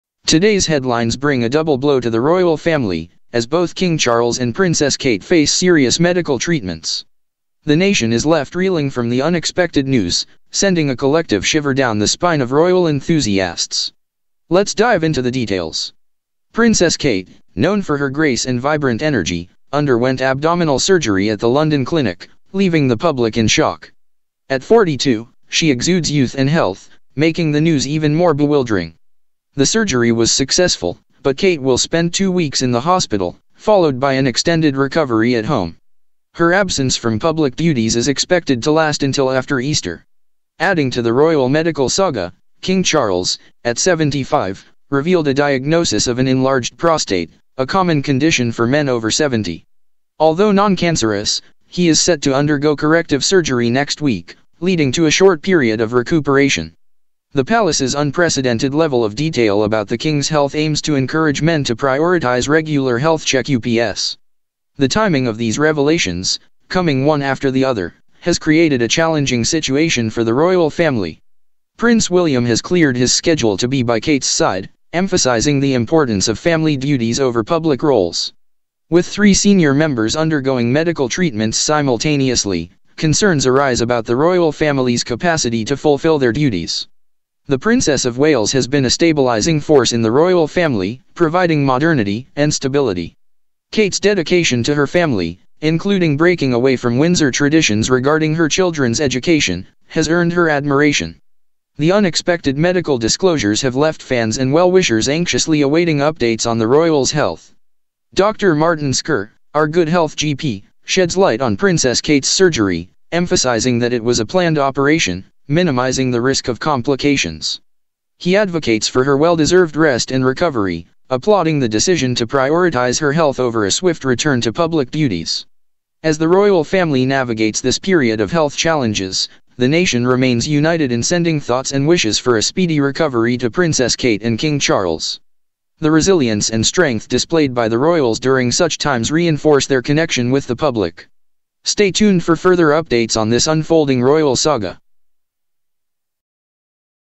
Double Blow for Royals: Are King Charles and Queen Kate Facing Serious Health Issues?
Today's headlines bring a double blow to the Royal Family, as both King Charles and Princess Kate face serious medical treatments. The nation is left reeling from the unexpected news, sending a collective shiver down the spine of royal enthusiasts. Let's dive into the details.